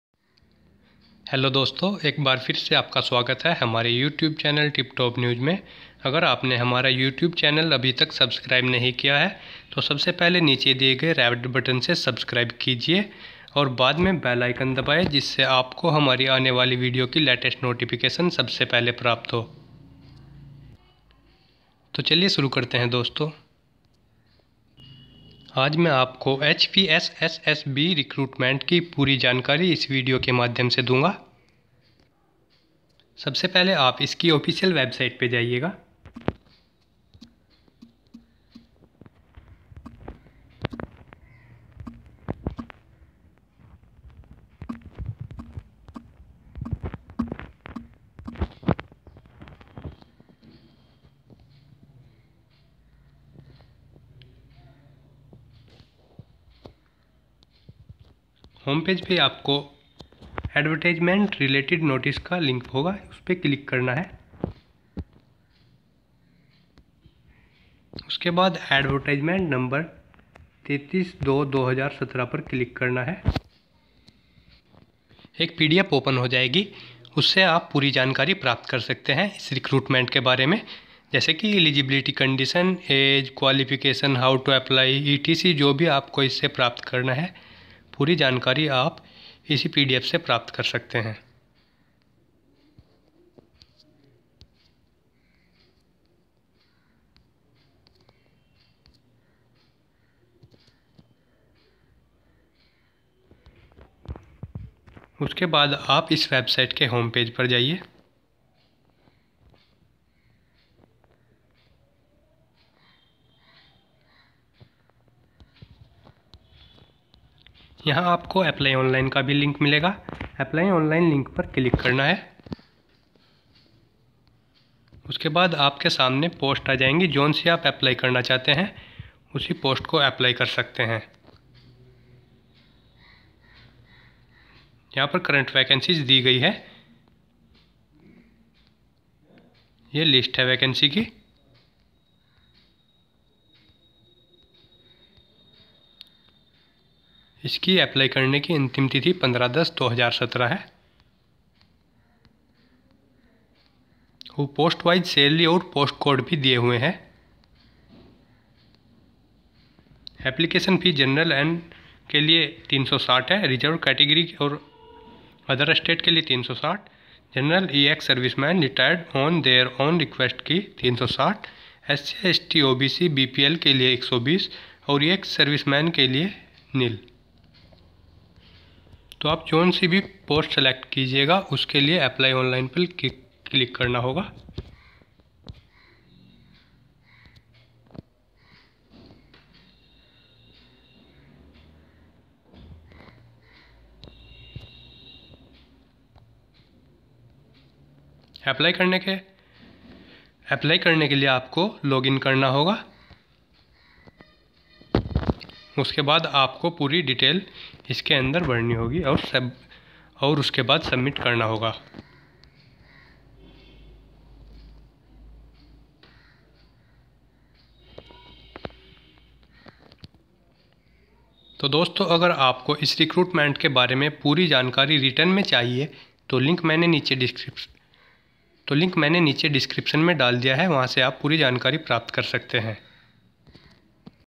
hpsssb.hp.gov.in HPSSSB Recruitment 2017 Govt Jobs In Himachal Pradesh (HP) Hamirpur
Himachal Pradesh Subordinate Services Selection Board (HPSSSB) has released a vacancy notification as “HPSSSB Hamirpur Recruitment 2017-18” for filling up 2945 vacancies of Sub Inspector, Radiographer, Librarian, Junior Engineer (JE), Driver and Others Posts. Candidates can fill online application form for HPSSSB Vacancy 2017 through official website that is from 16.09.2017 to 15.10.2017.

Check all info regarding HPSSSB Jobs 2017 such as qualification, how to apply etc through this

Important Dates -
The online application can be filled from: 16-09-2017
Last date to submit online application is: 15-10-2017 till 11.59 PM